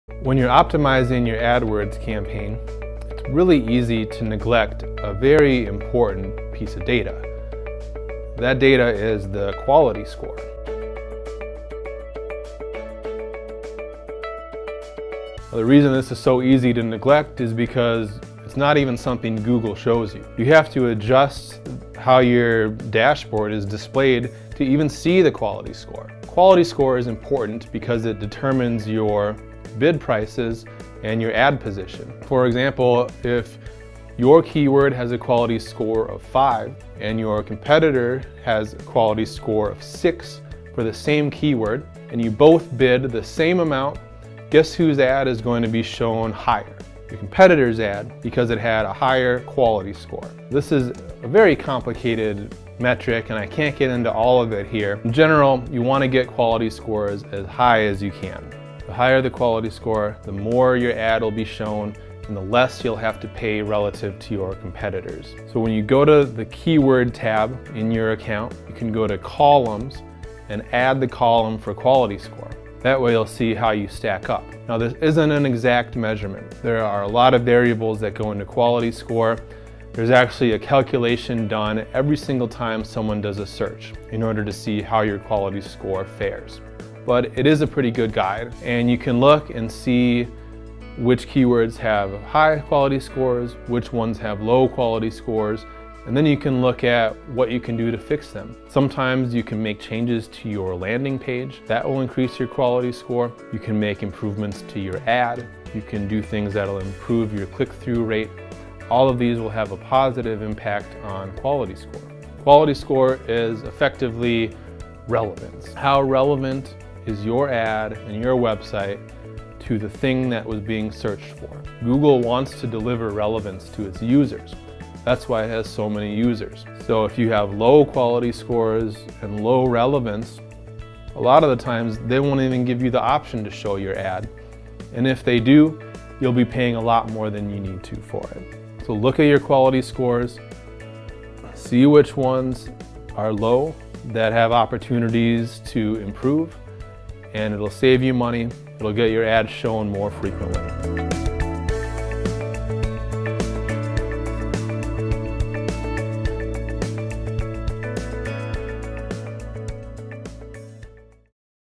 AdWords Mistake: Ignoring Quality Score
Video Transcript:

When you’re optimizing your AdWords campaign it’s really easy to neglect a very important piece of data. That data is the quality score.

The reason this is so easy to neglect it’s not even something Google shows you by default. You have to adjust how your dashboard is displayed to even see the quality score.

Quality score is important because it determines your bid price and and your ad position. For example, if your keyword has a quality score of 5 and your competitor as a quality score of 6 for the same keyword and you both bid the same amount, guess whose ad is going to be shown higher? The competitor’s ad because it has a higher quality score.

This is a very complicated metric and I can’t get into all of it here. In general you want to get quality scores as high as you can. The higher the quality score, the more your ad will be shown, and the less you will have to pay relative to your competitors.

So when you go to the Keywords tab in your account you can go to “Columns” and add the column for quality score. That way you will see how you stack up.

This isn’t an exact measurement. There are a lot of variables that go into quality score. There’s actually a calculation done every single time someone does a search in order to see how your quality score fares. But it is a pretty good guide and you can look and see which keywords have high quality scores, which ones of low quality scores. And then you can look at what you can do to fix them. Sometimes you can make changes to your landing page that will increase your quality score. You can make improvements to your ad. You can do things that will improve your click through rate. All of these will have a positive impact on quality score.

Quality score is effectively: relevance. How relevant is your ad or your website to the thing that was being searched for. Google wants to deliver relevance to its users – that’s why it has so many users. So if you have low quality scores and low relevance a lot of the time they won’t even give you the option to show your ad, and if they do you’ll be paying a lot more than you need to for it.

So look at your quality scores, see which ones are low that have opportunities to improve. It will save you money and it will get your ads shown more frequently.